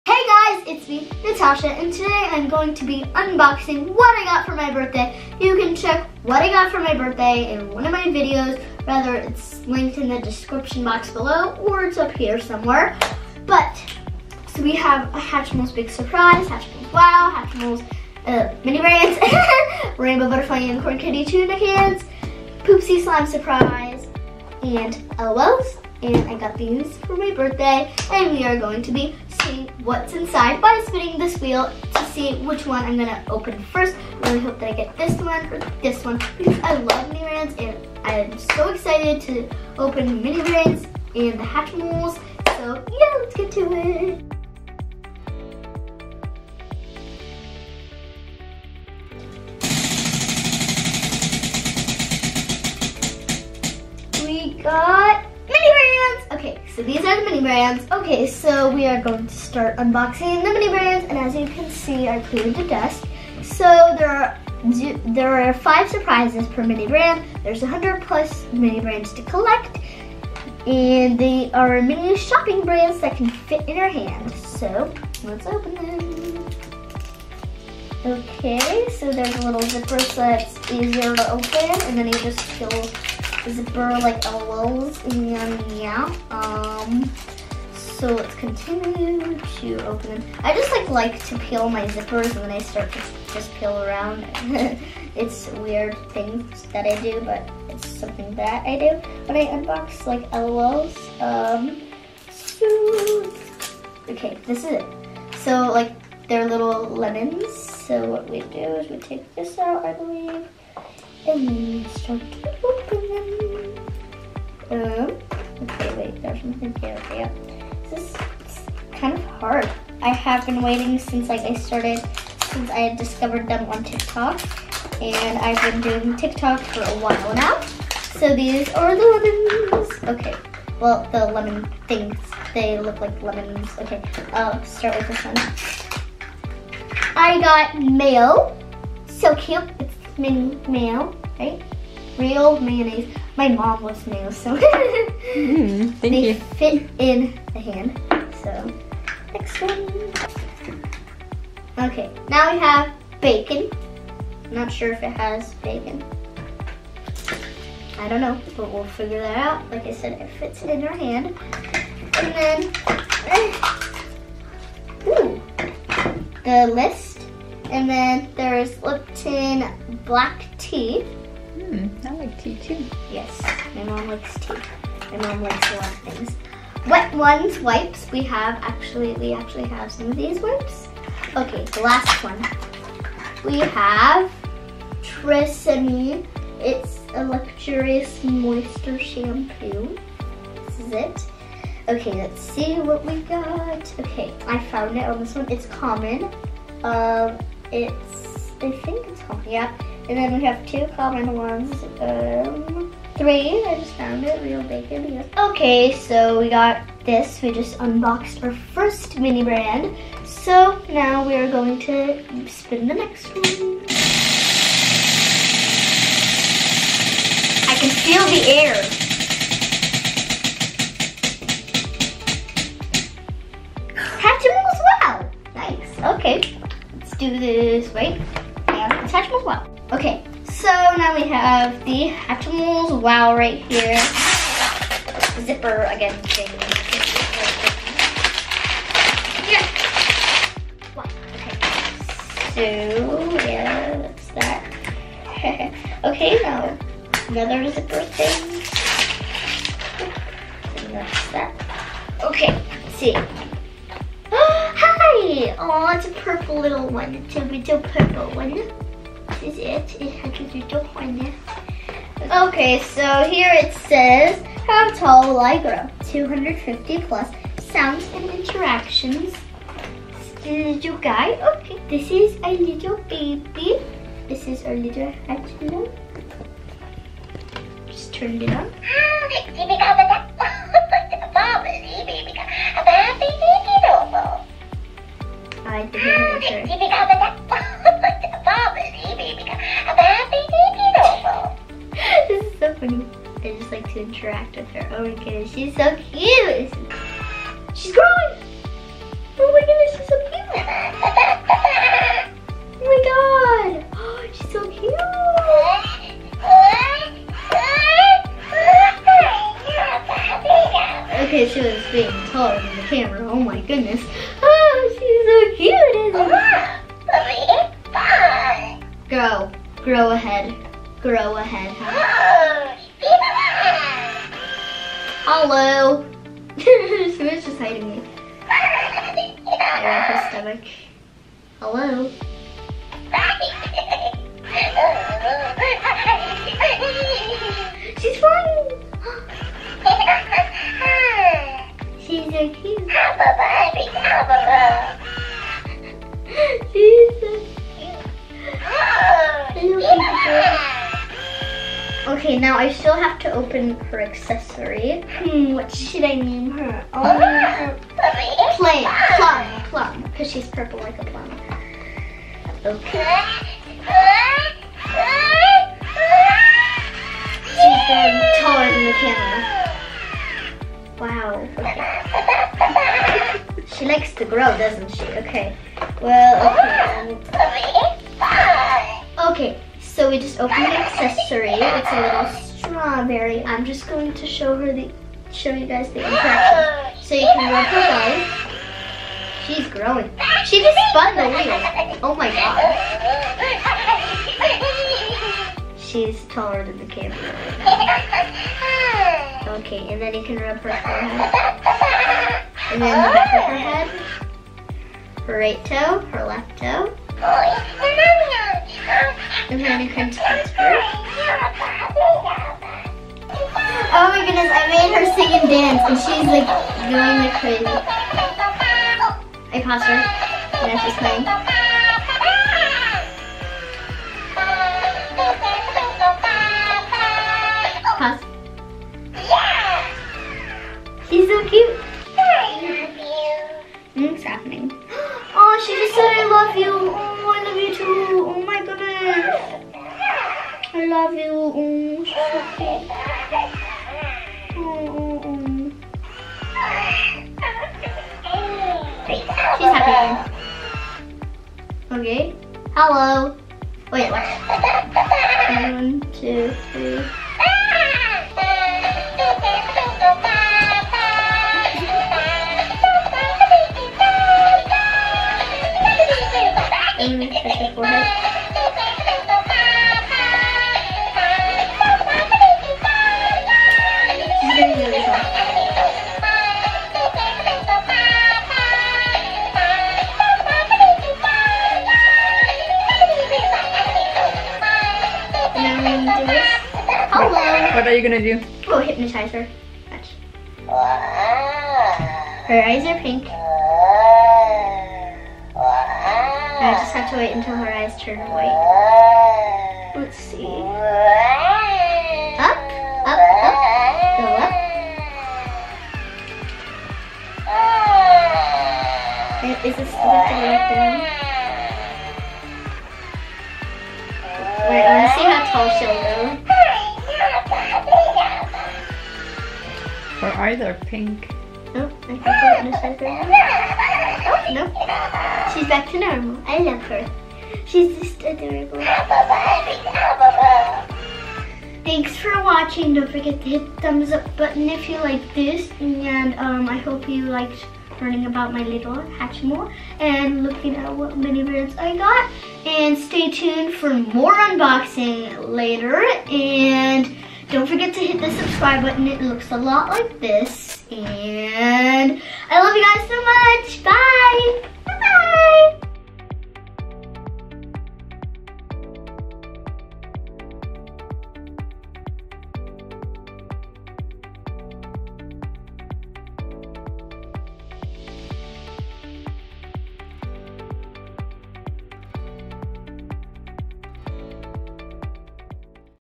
Spinning the MYSTERY Wheel & UNBOXING Whatever BIRTHDAY GIFT It Lands On!
Hi Guys,
Today, I am spinning the Wheel to pick which birthday gift I am unboxing first! If you miss my previous video, go check it out to see what I got for my birthday!
Thank you for stopping by and I hope you enjoy watching my video.

L O V E YOU GUYS! ❤️
💋's - Natasha

🖱🙌  LET'S HIT 1000 YAAAYYY!!!

Be you - you're Fabulous!